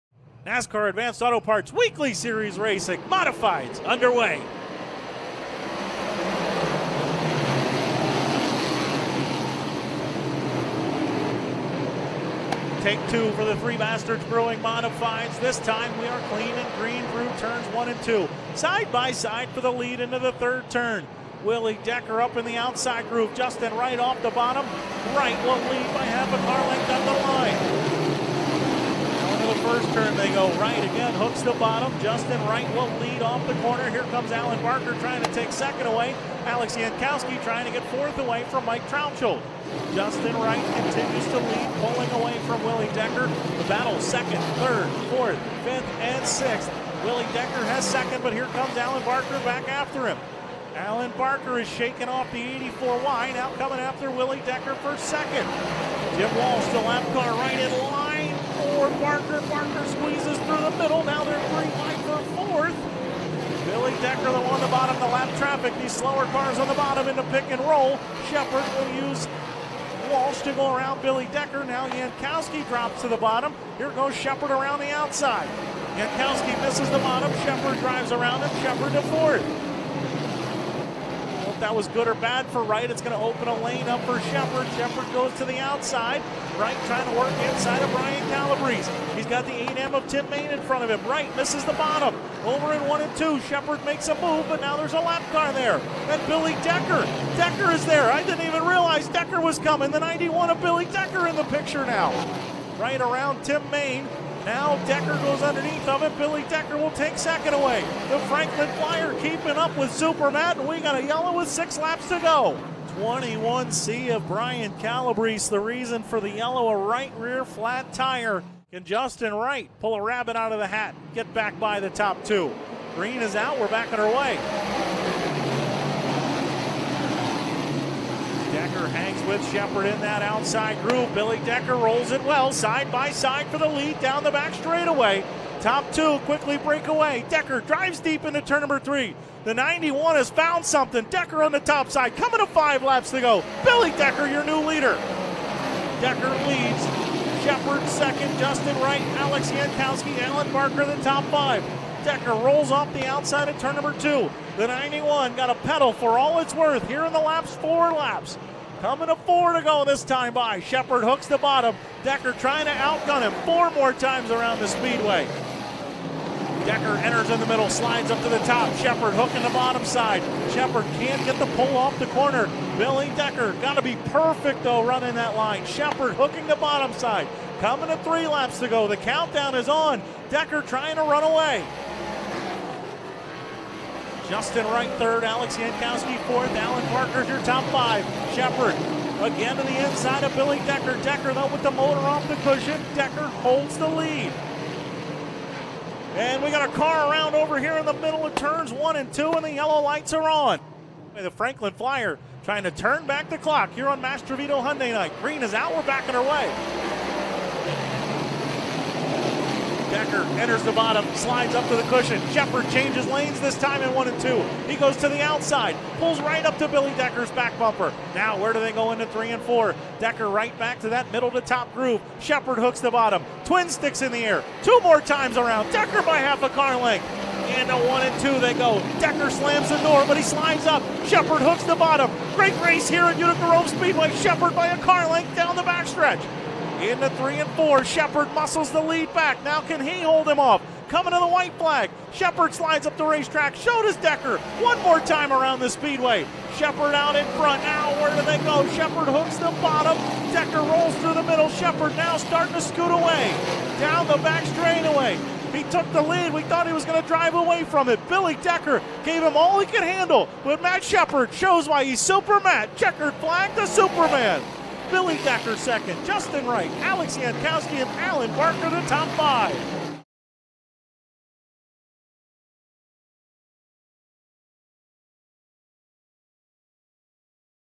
Race Highlights: 2023 Big Block Modifieds at Utica-Rome Speedway
Watch the Big Block Modifieds feature highlights at Utica-Rome Speedway from July 7, 2023 on FloRacing.